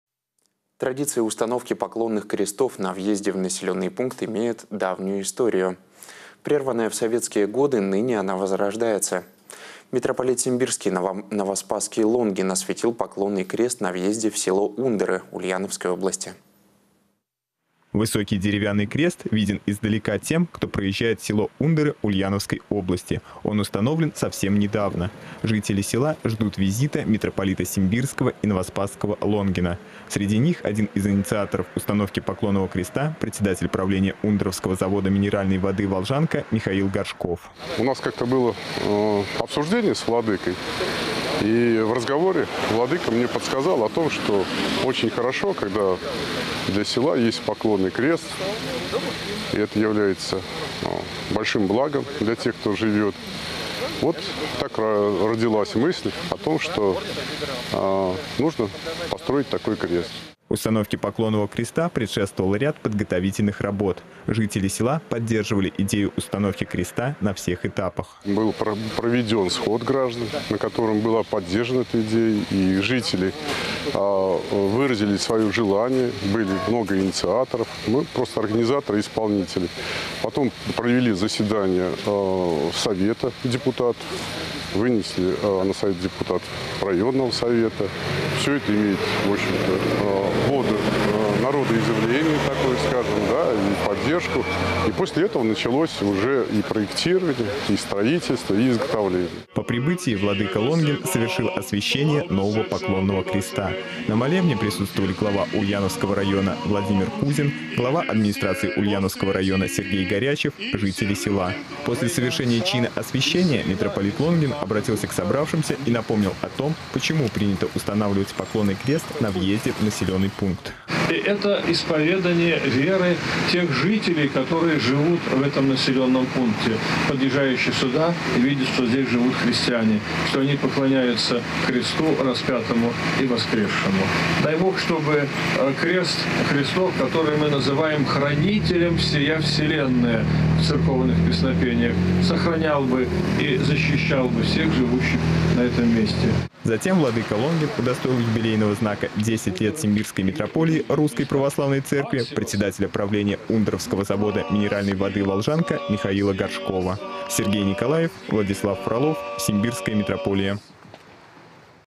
Митрополит Симбирский и Новоспасский Лонгин освятил поклонный крест на въезде в село Ундоры
Традиция установки поклонных крестов на въезде в населенный пункт имеет давнюю историю. Прерванная в советские годы, ныне она возрождается. Митрополит Симбирский и Новоспасский Лонгин освятил поклонный крест на въезде в село Ундоры Ульяновской области.